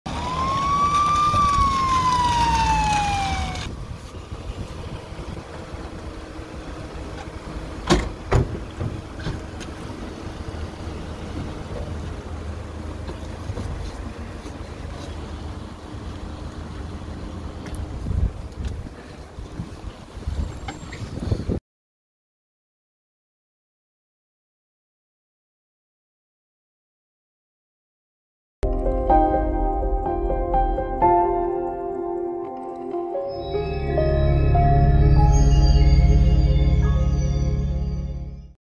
Njëri kishte armë me vete, tjetri lëndë narkotike në makinë, arrestohet 22-vjeçari në Shkodër
reportTV lajmeshqip lajme Gjatë kontrollit të mjetit, i cili u braktis nga drejtuesi, u gjet një armë zjarri pistoletë, e cila i përkiste pasagjerit 22-vjeçar me iniciale B.C, që u arrestua, po ashtu u zbulua dhe një sasi lënde narkotike e llojit kanabis.